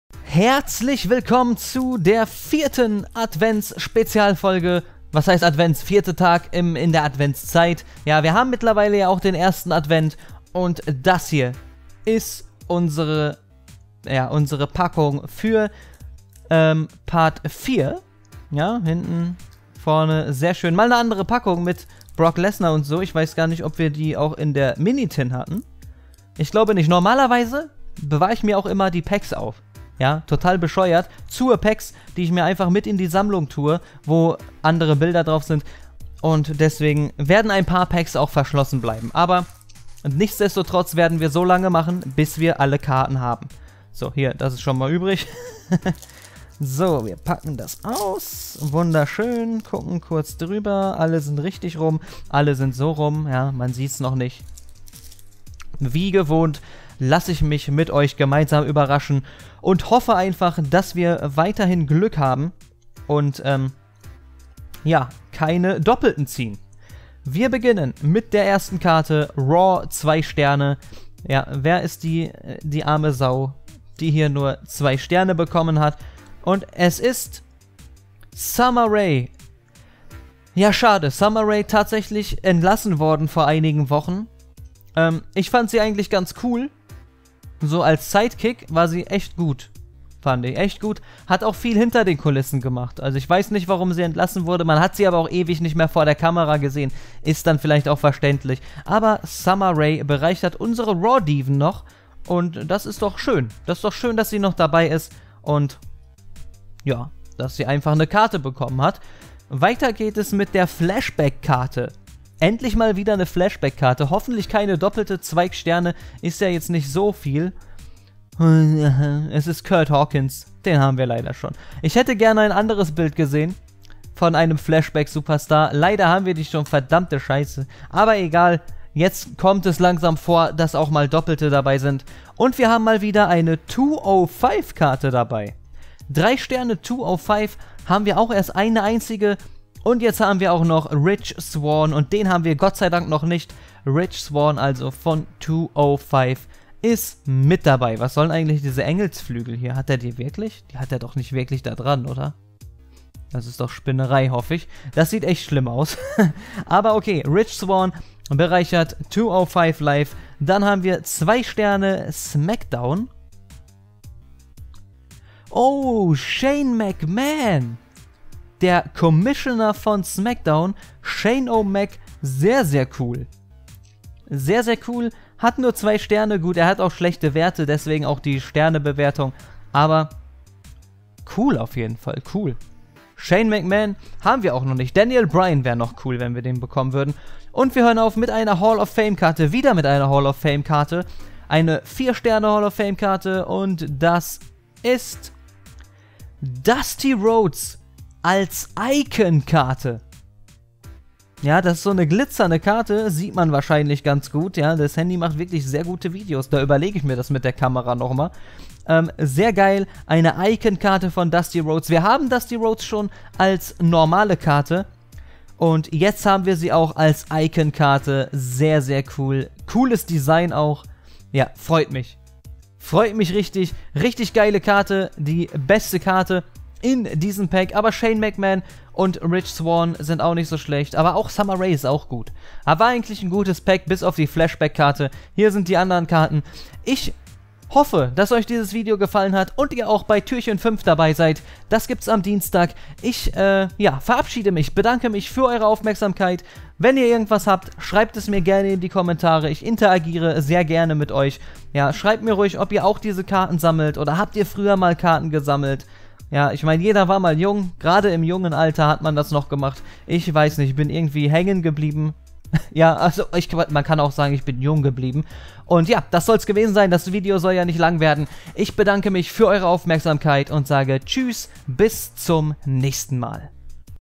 Collectable - Slam Attax 10th Anniversary - Adventskalender Türchen 4
Wir sammeln gemeinsam ^^
Die Slam Attax 10th Anniversary Auflage ist seit November 2017 in Deutschland erhältlich. Unter der Rubrik Collectables machen wir die Sammlung voll :)